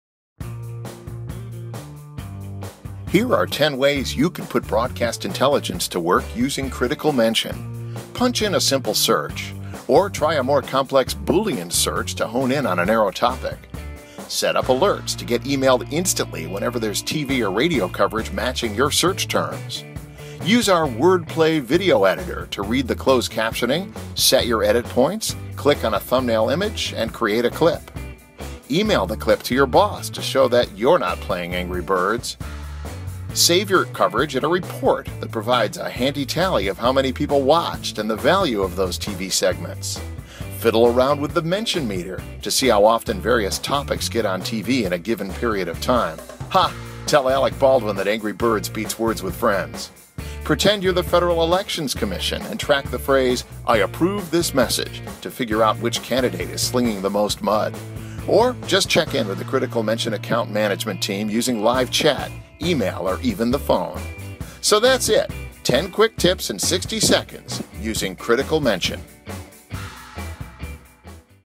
10 Tips for Using Critical Mention
This video is a 60-second tutorial showing how the Critical Mention platform can be used to search for TV and radio coverage, create alerts, edit coverage, share it within your organization, build reports and perform competitive intelligence research.